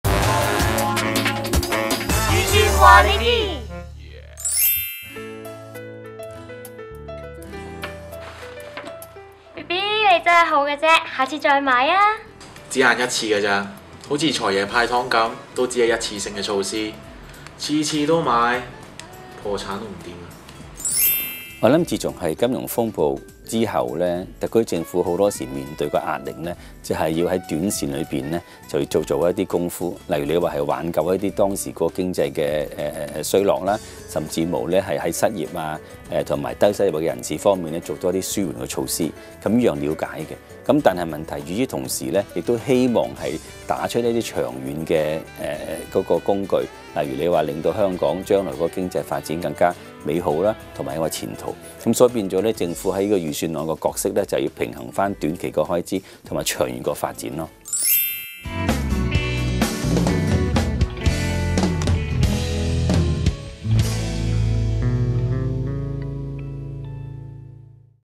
通識一分鐘《預算話你知》一次性紓困措施？
近年預算案均會提出一次性紓困的措施，理據為何？為何不提出長遠措施？